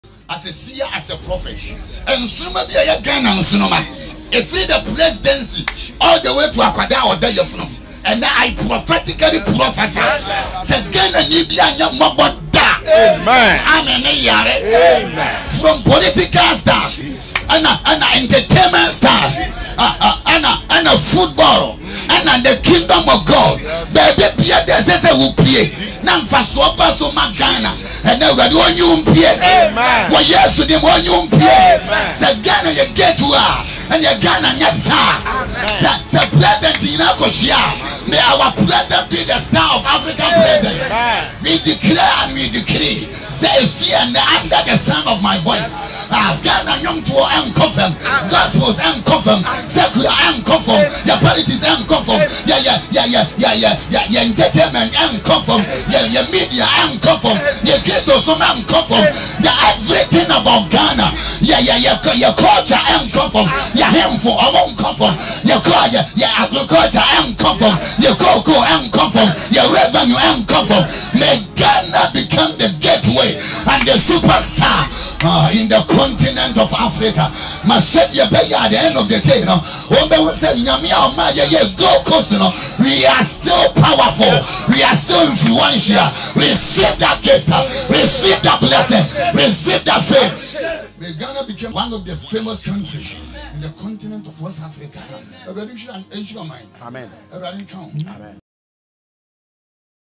PRAYERS FOR THE NATION GHANA FROM APOSTLE FRANCIS AMOAKO ATTAH
I prophetically pray for this nation on this day that May Ghana become a gateway for the continent of Africa. I am proud to be a Ghanaian. God bless Ghana. Amen